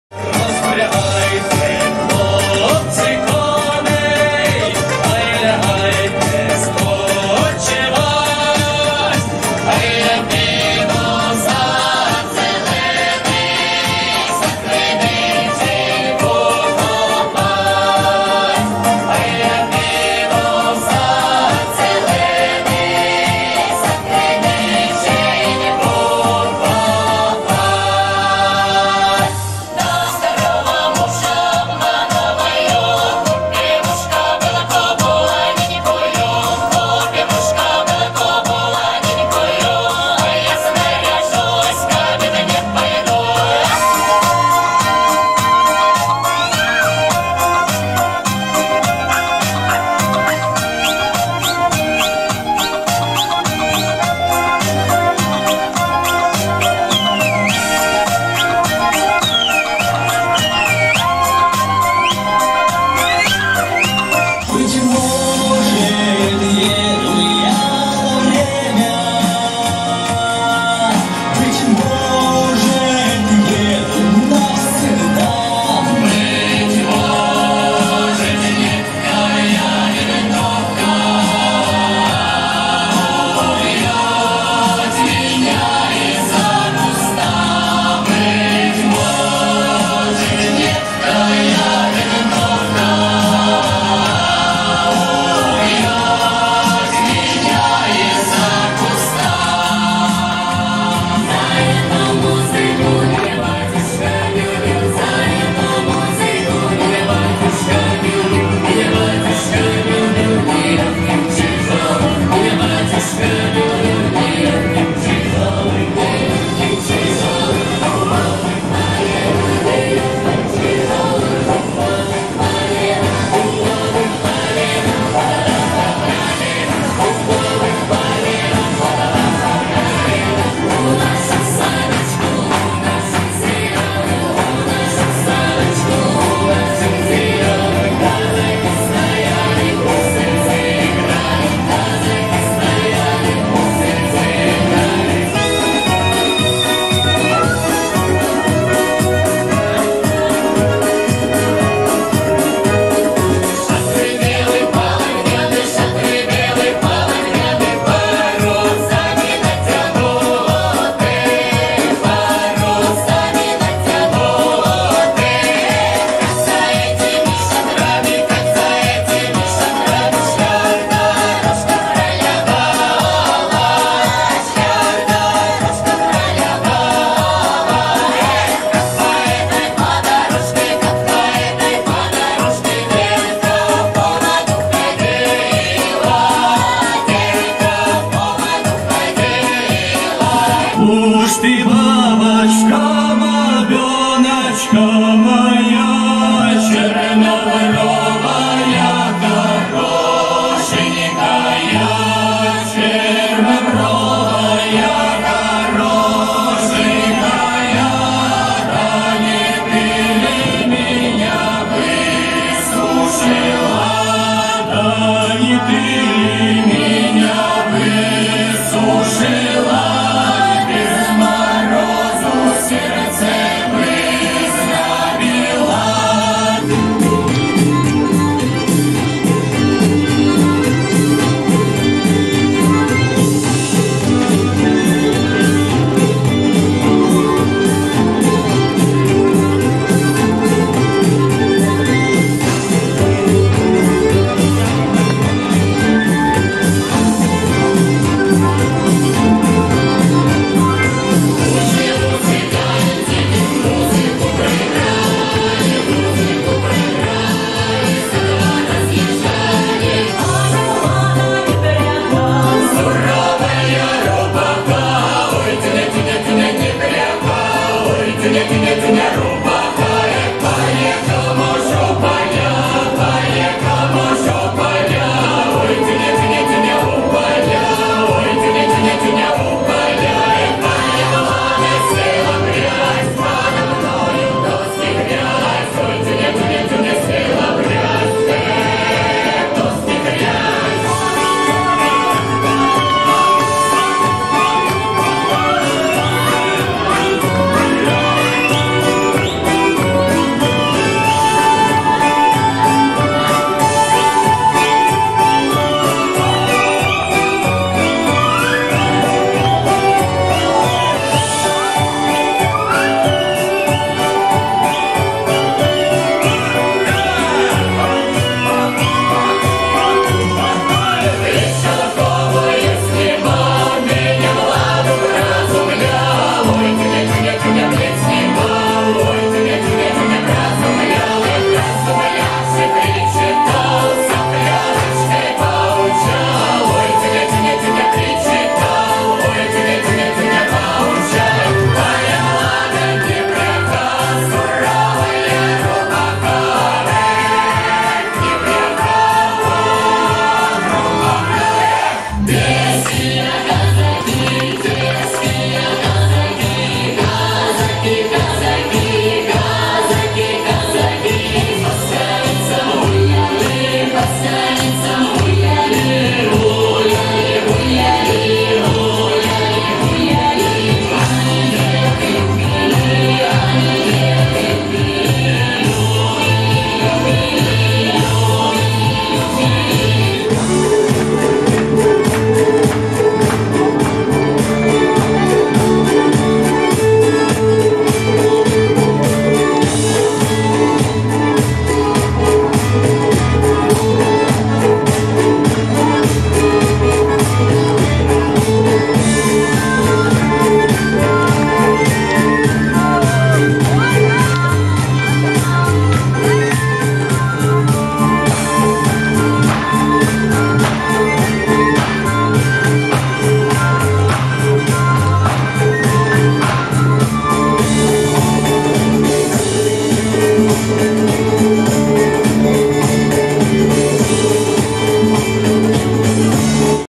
Ансамбль "Наследие" (микс)
Фрагменты праздничного концерта вокального ансамбля песни терских казаков "Наследие" (художественный руководитель - Лариса Дёгтева) концертно-творческого объединения "Аккорд" на сцене  "Ставропольца" 7 мая 2021 г.